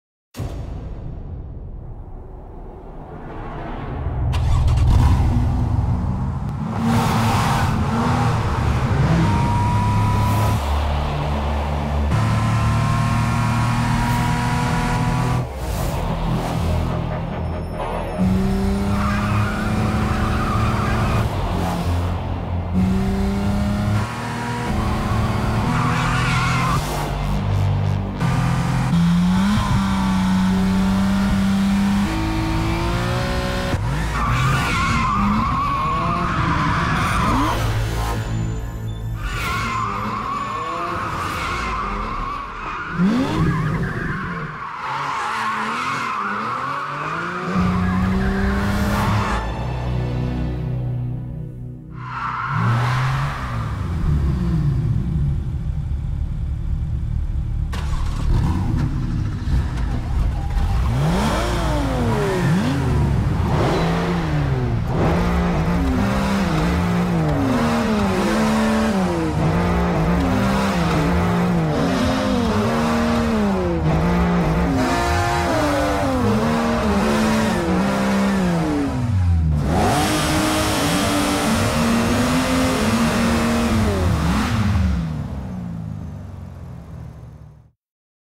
7 nesil Porsche 911 egzoz sesleri ile müzik senfonisi çalma
Mercedes-Benz X 250 d 4MATIC test sürüşü videomuzu izlediniz mi?